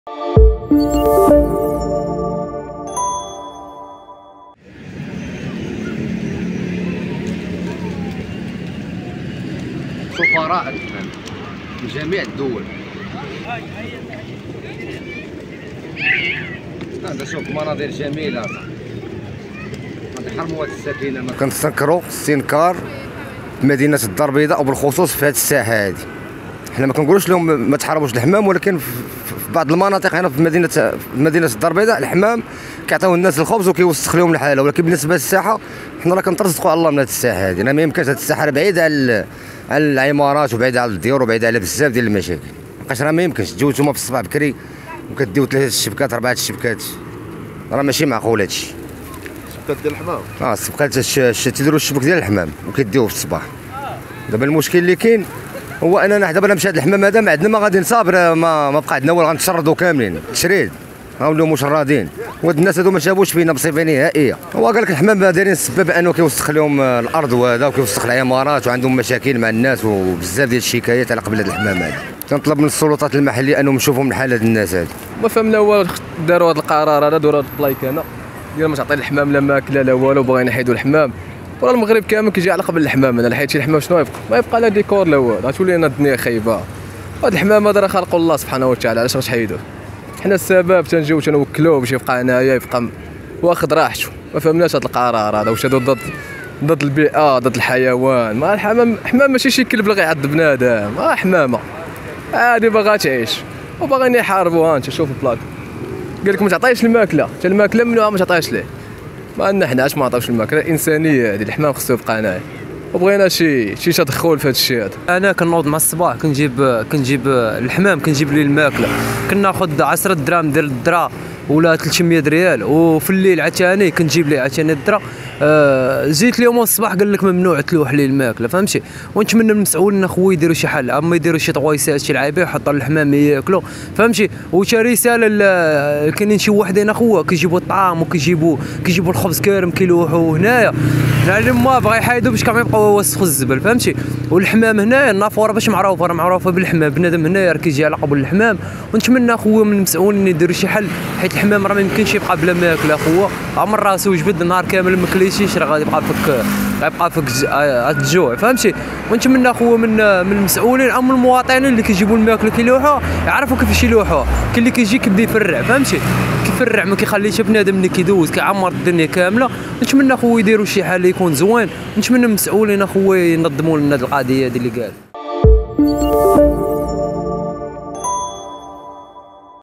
من يسعى إلى تجويع الحمام بساحة محمد الخامس بالدار البيضاء؟
انتقد بعض المواطنين مسألة منع تقديم الأكل للحمام بساحة محمد الخامس بالبيضاء، والتي تشتهر بالحمام سواء في نسختها الأولى أو الثانية